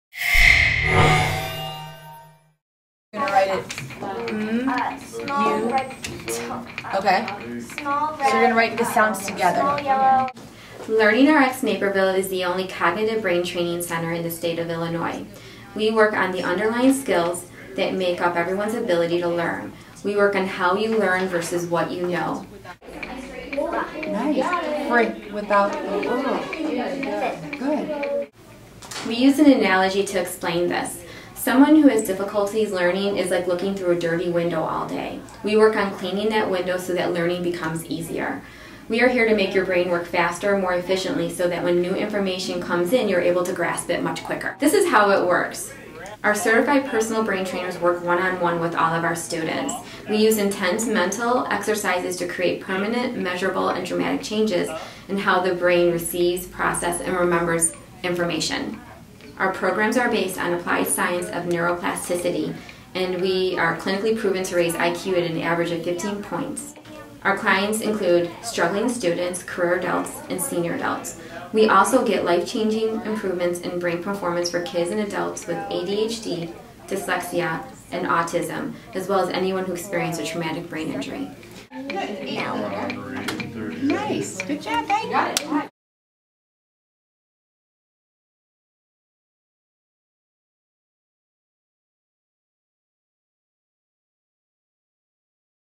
Brain training for kids in Naperville
Mia Tischer, executive director/owner of the LearningRX in Naperville, discusses how the center offers personalized instruction that can help people of all ages sharpen their mental skills.